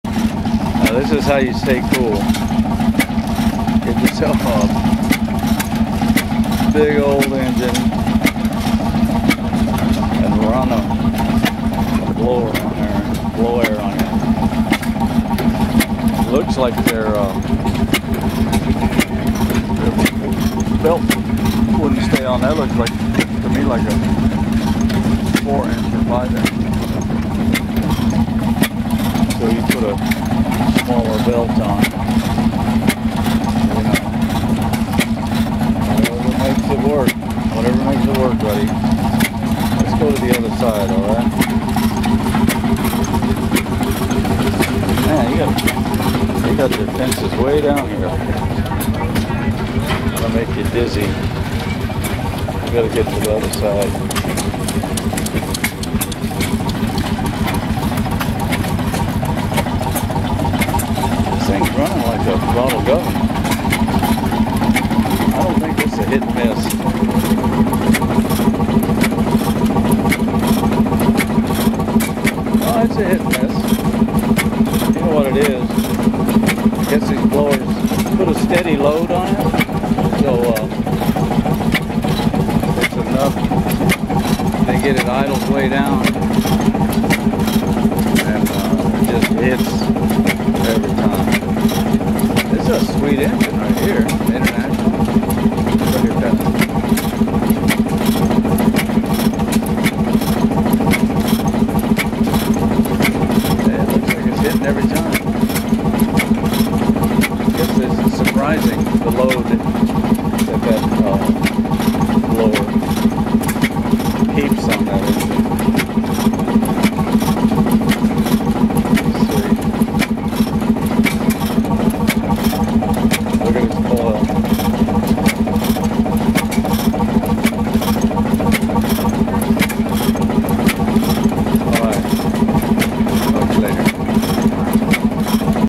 nice old hit and miss engine running a blower
Gotta keep cool in the heat of Plymouth Ca. at the end of July 2021.